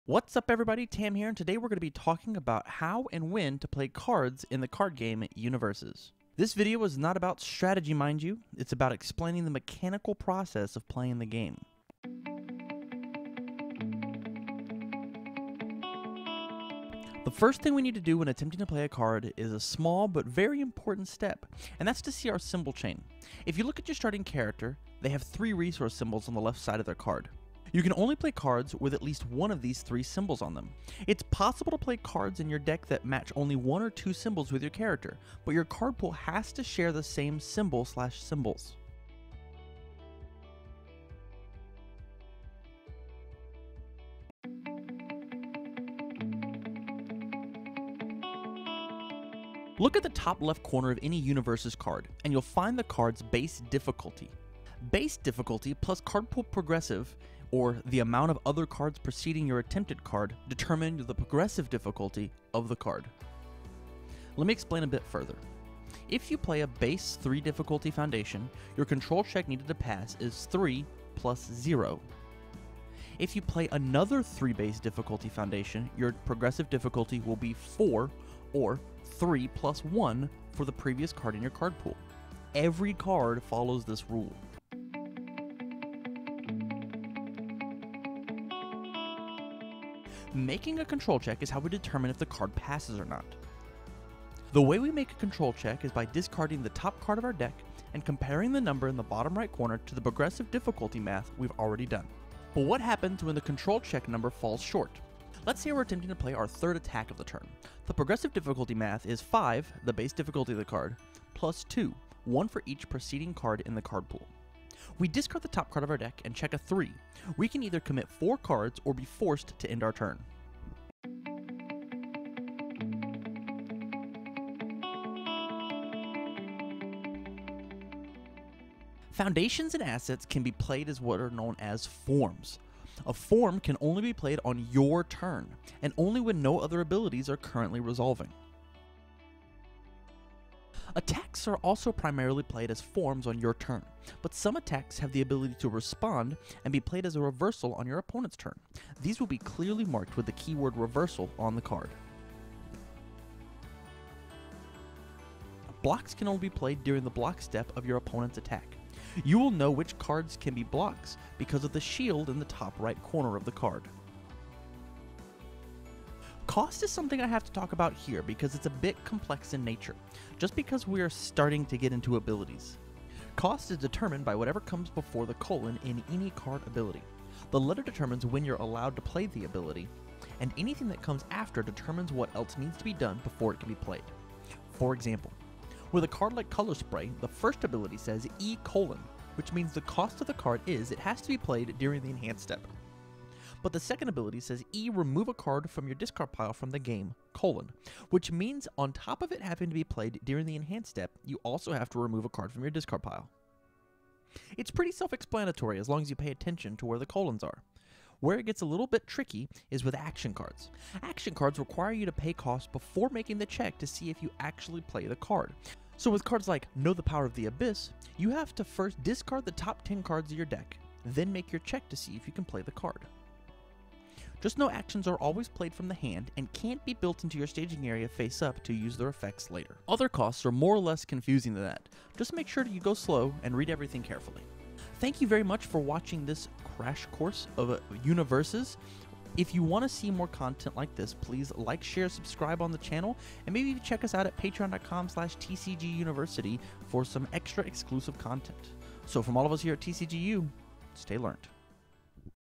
HOW TO PLAY CARDS - UniVersus Crash Course | TCGU
This Video is a look at HOW TO PLAY CARDS in UniVersus.